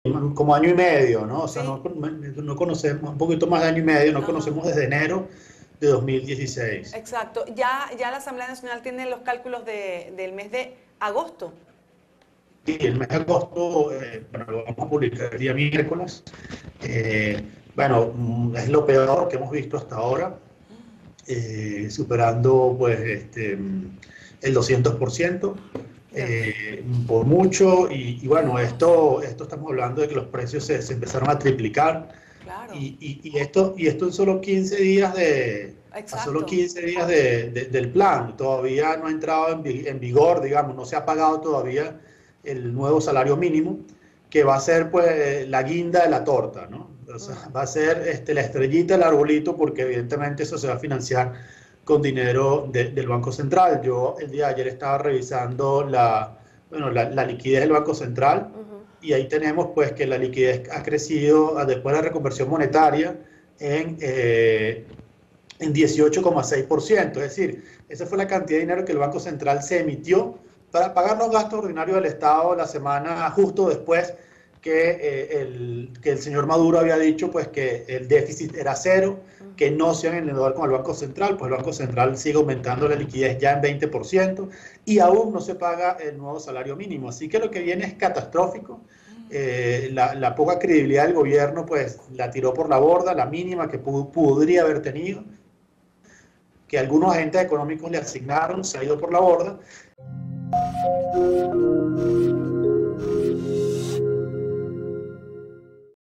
Medidas económicas triplicaron los precios
El diputado a la Asamblea Nacional, Ángel Alvarado, informó que la inflación del mes de agosto alcanzó 200%. Además, la liquidez del Banco Central creció en casi 20%.